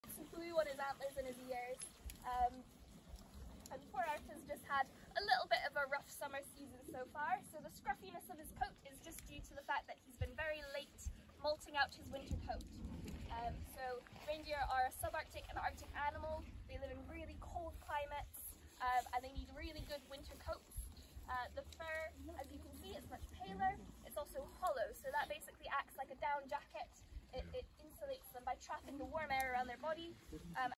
Walking with Reindeer
Walking with Reindeer at the Reindeer Park on Cairn Gorm, Cairngorm National Park, Speyside, Scotland